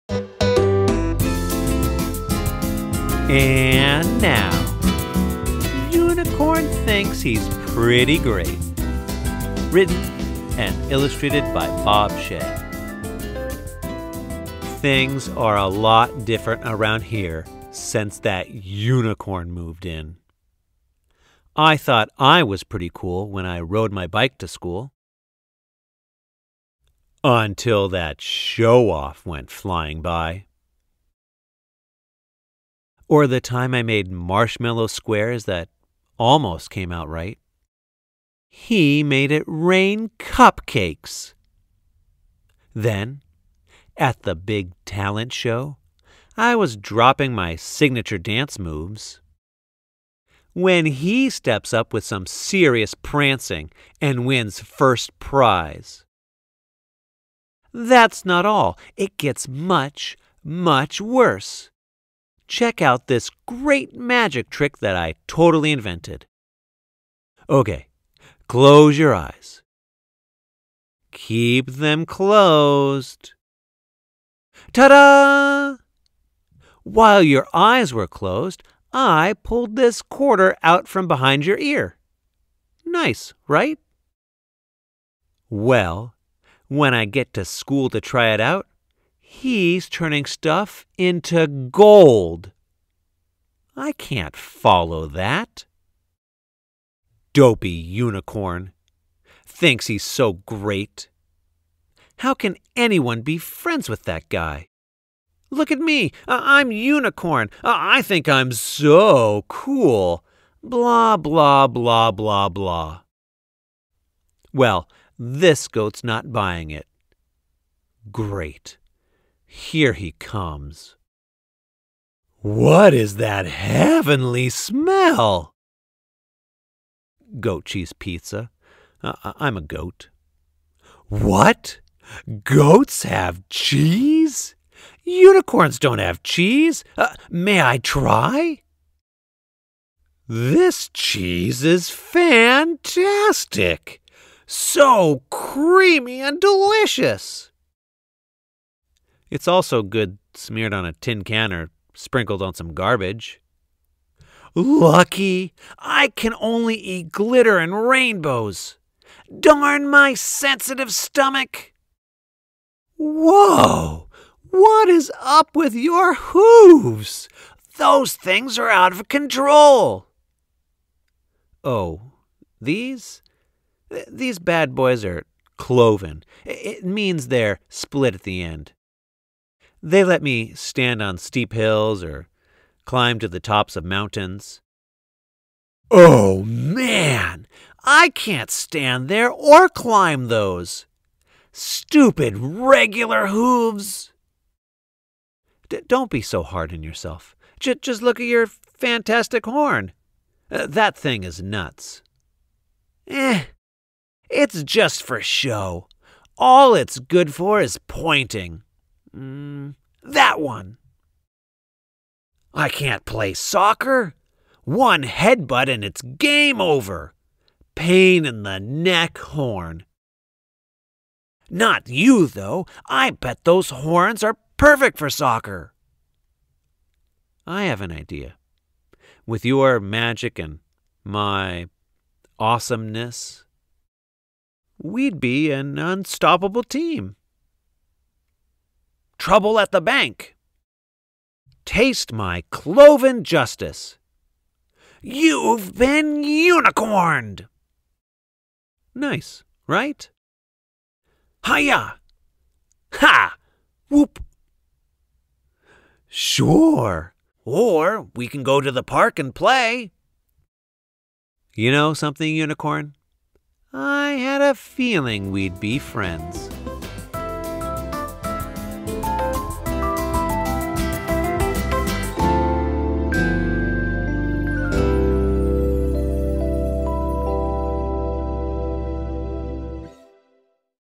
Unicorn Thinks He’s Pretty Great by Bob Shea
Goat isn’t happy Unicorn is around. Turns out Unicorn is pretty jealous of some of the things Goat can do. Written and illustrated by Bob Shea.